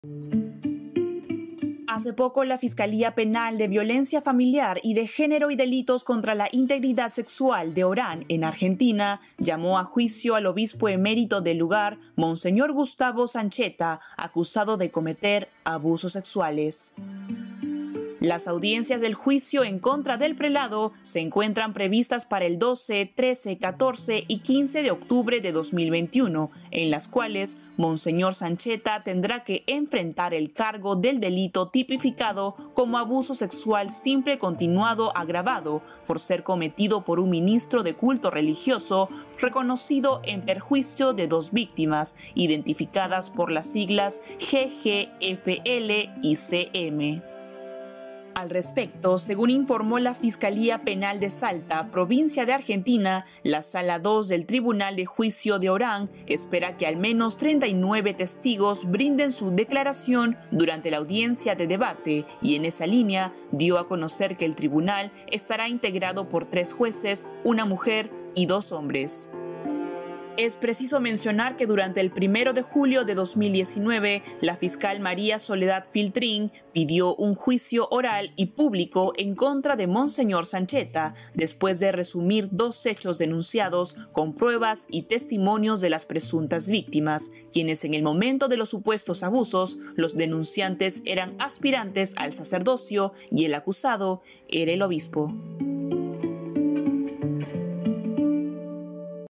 Fiscalía llama a juicio a Obispo acusado de abusos sexuales y de poder
Fiscalía argentina llamó a juicio a Obispo acusado de abusos sexuales.